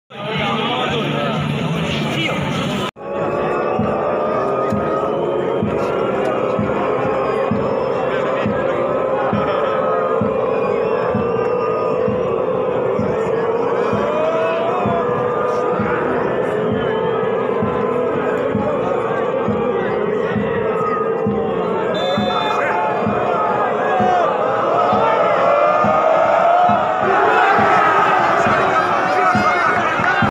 Дублёры не смогли удержать победу в Черкизово. Незабитый пенальти
14.04.23, М-Лига, Локомотив - Спартак 1:1
Артём Быковский не смог реализовать пенальти